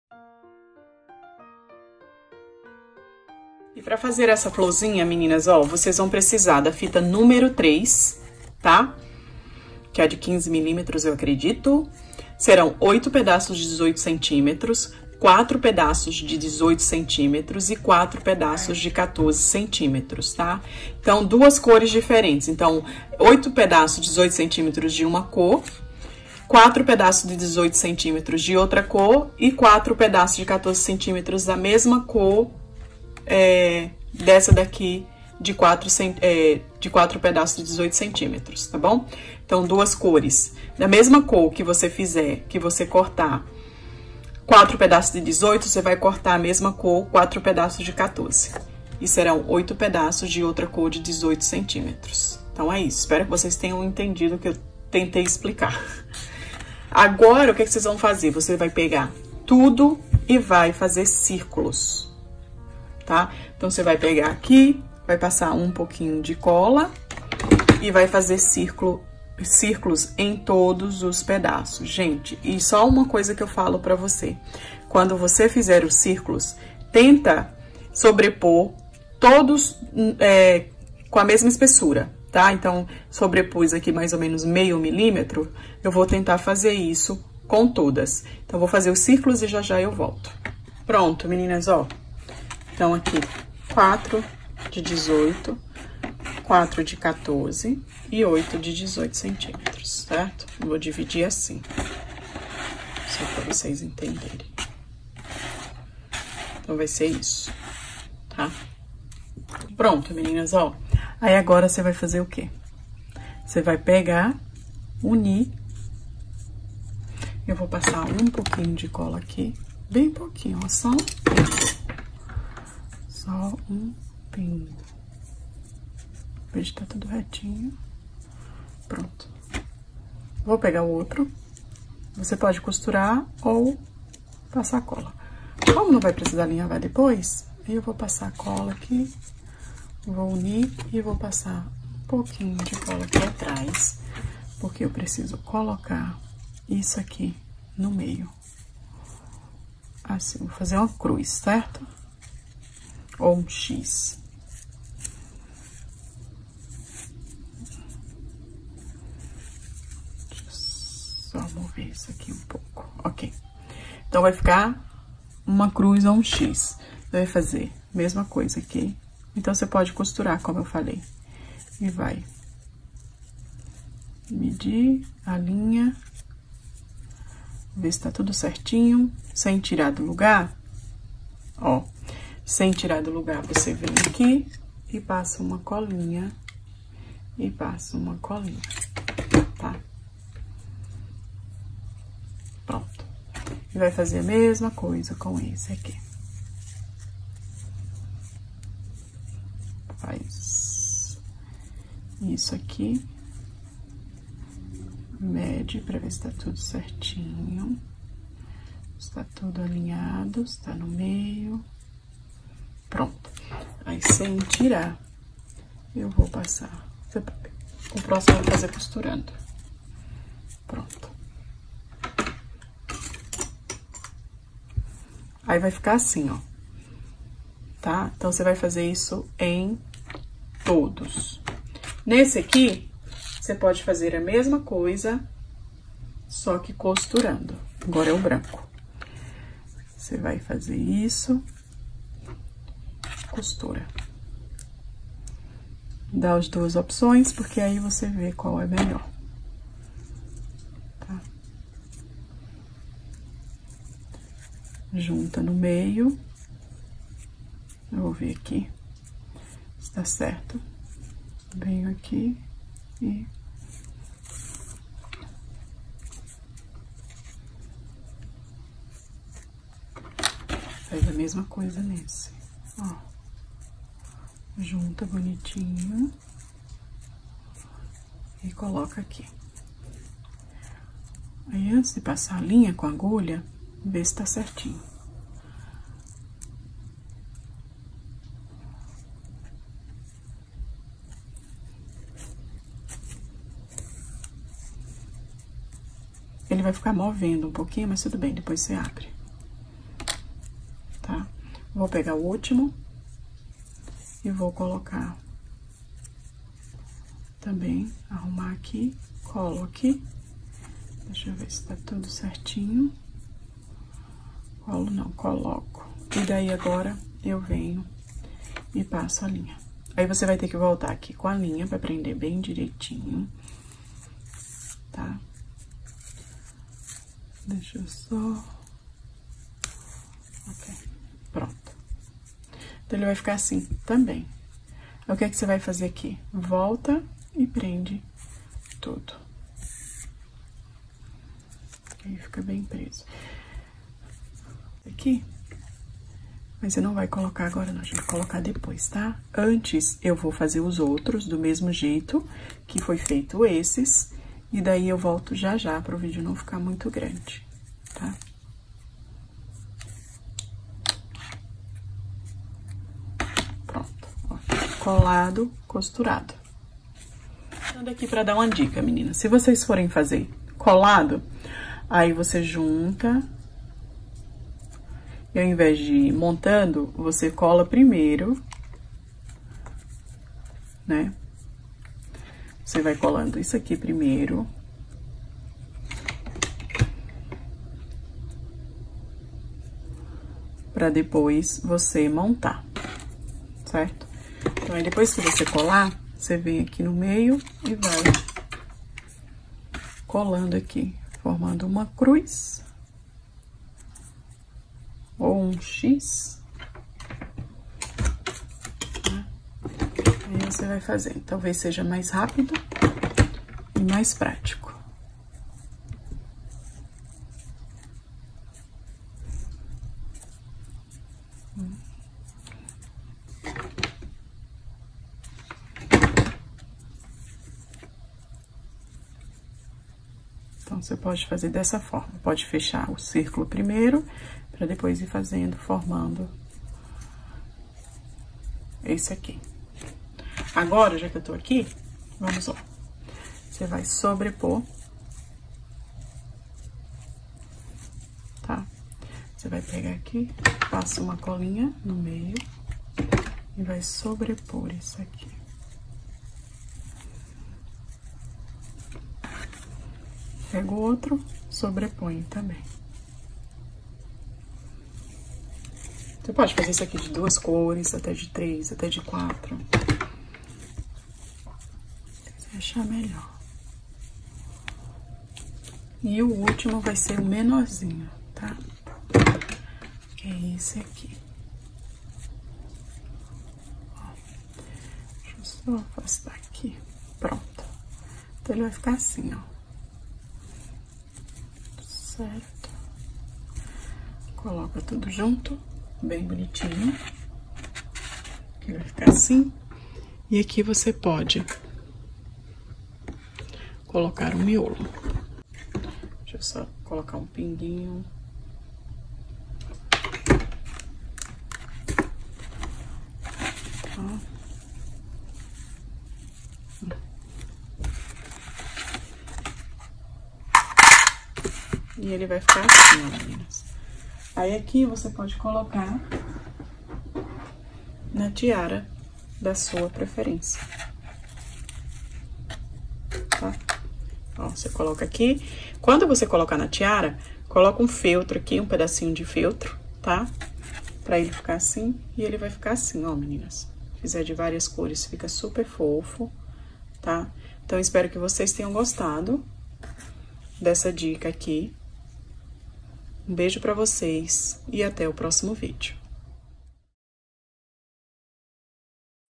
Flor de fita super fácil
Materiais

Fita n3 - 15mm ribbon

8x18cm
4x18cm
4x14cm
Cola quente
Miolo e aplique da sua preferência